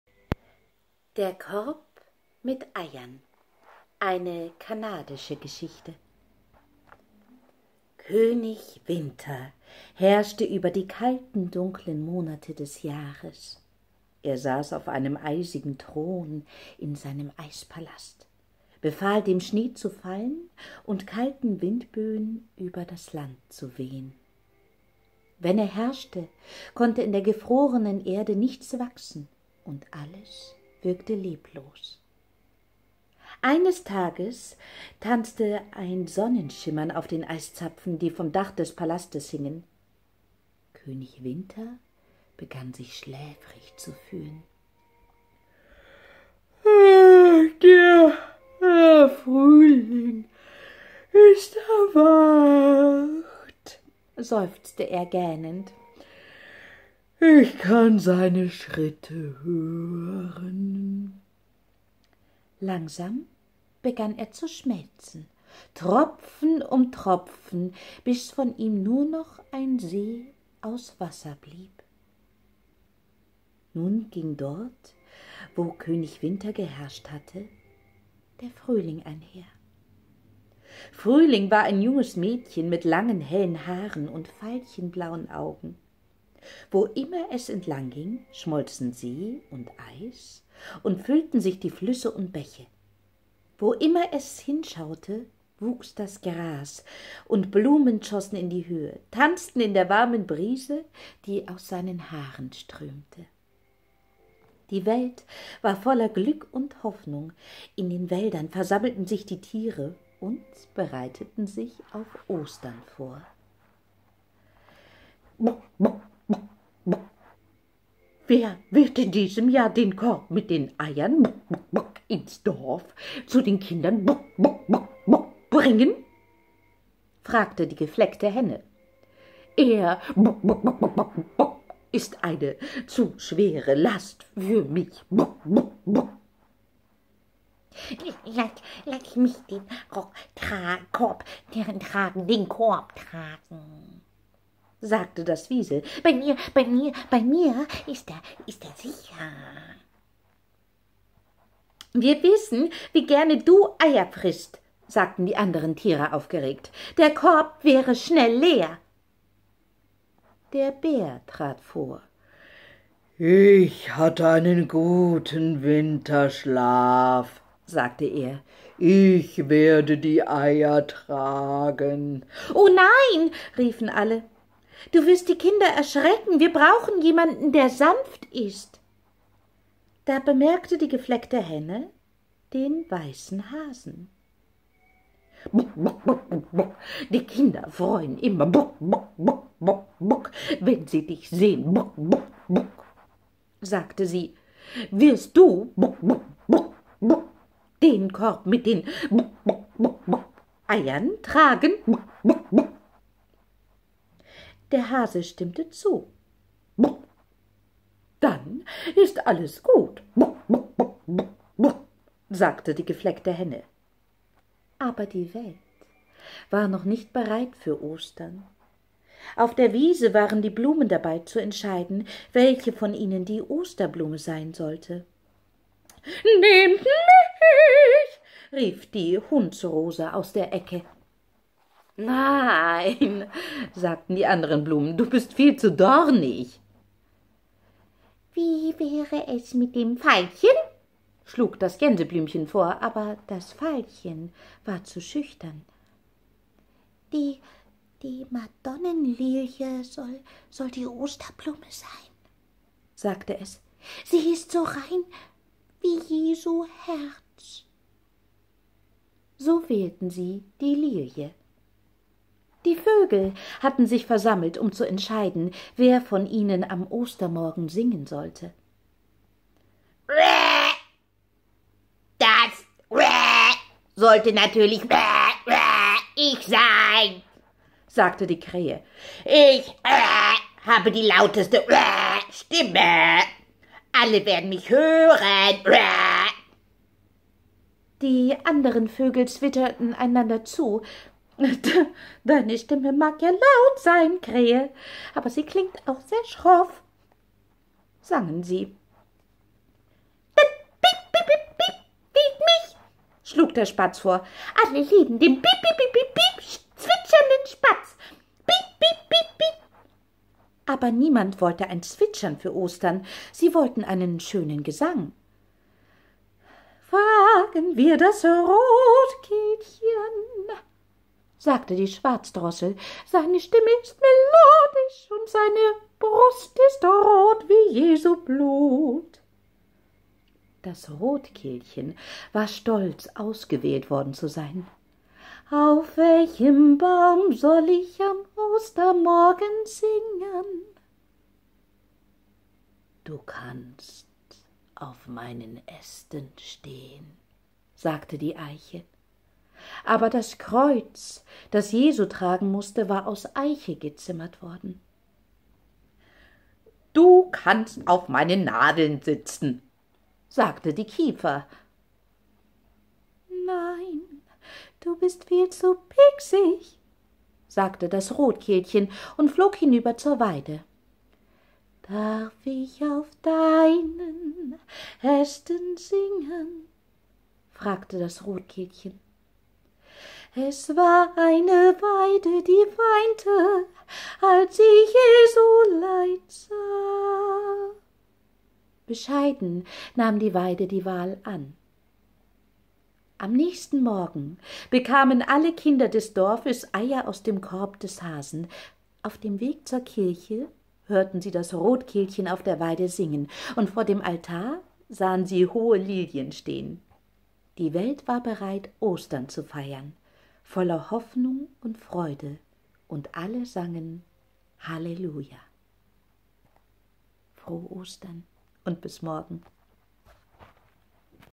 DER KORB MIT EIERN
FROHE OSTERN ihr Lieben!!!

Meine Ostergeschichte kommt aus Kanada!

Habt viel Spaß und ich wünsche euch allen Frieden,  Hoffnung, Glück und Gesundheit💙💚💛🧡❤💜